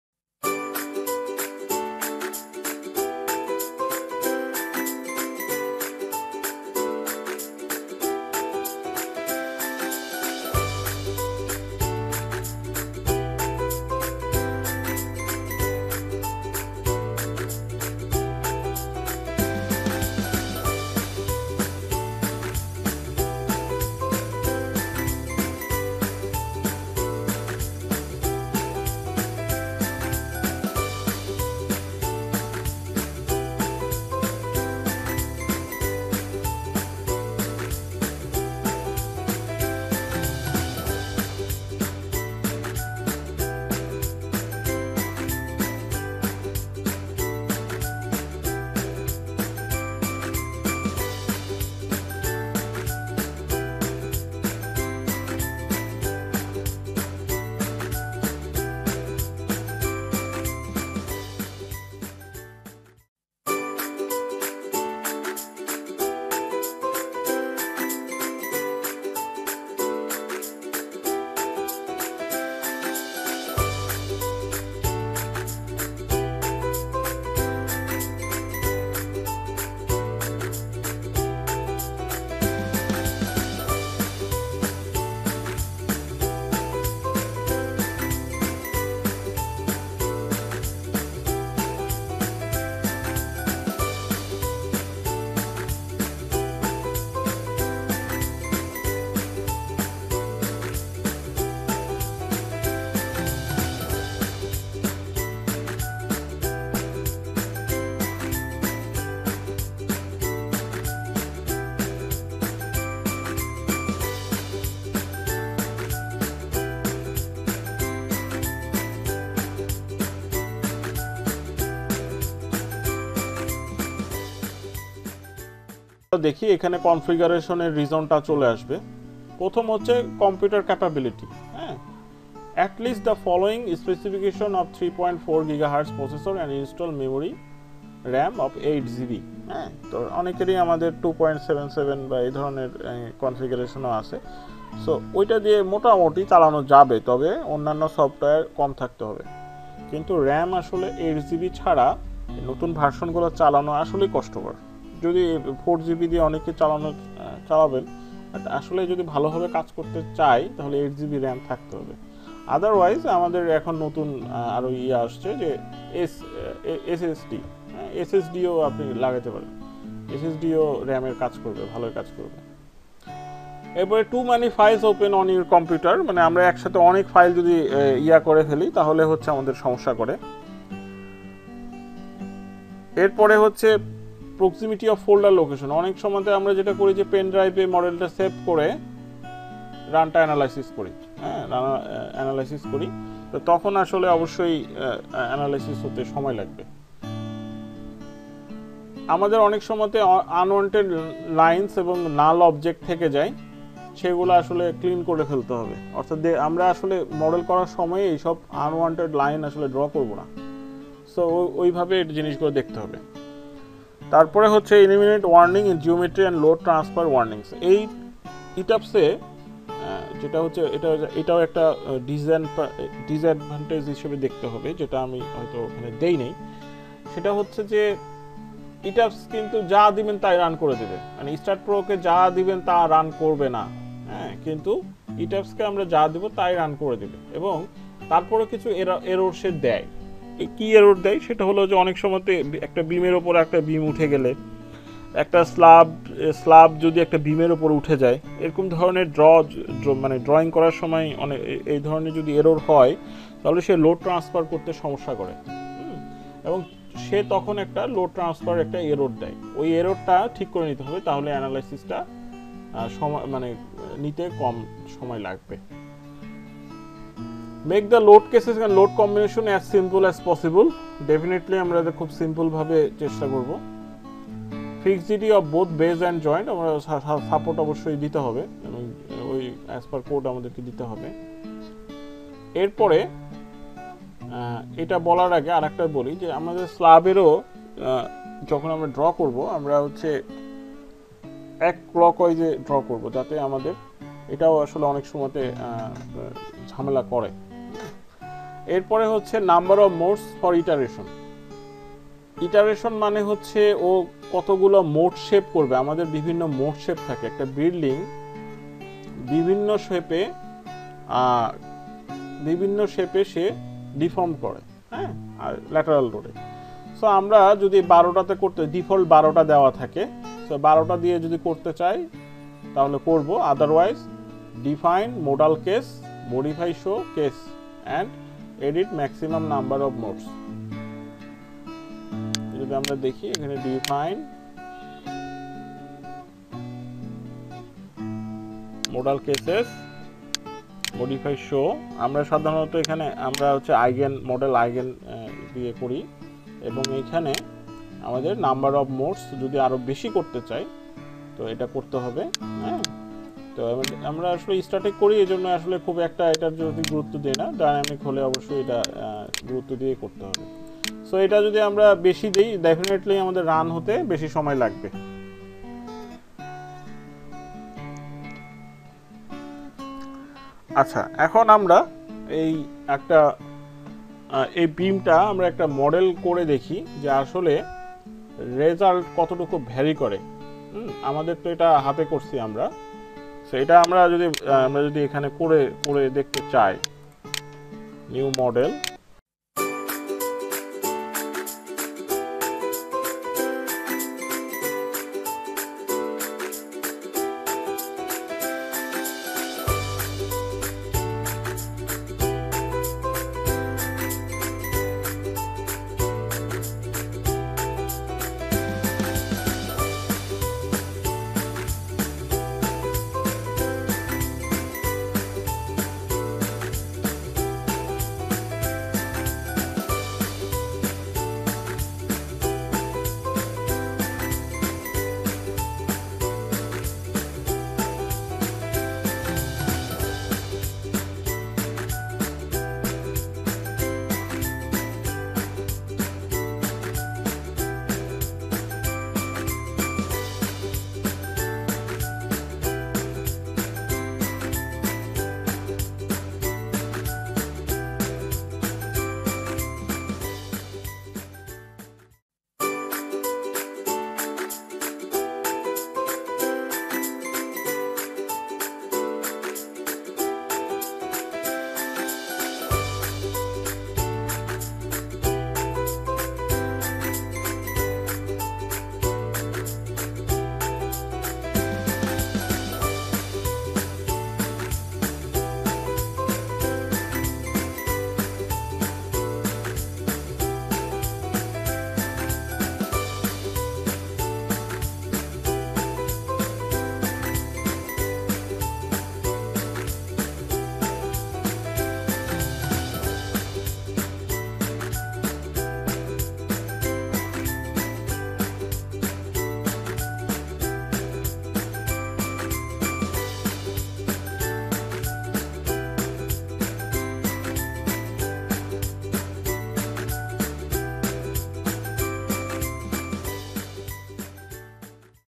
etabs run analysis problem
Etabs is a very popular software for structural analysis.

00:00 Introduction
02:06 Run Analysis Problem
05:33 Model Example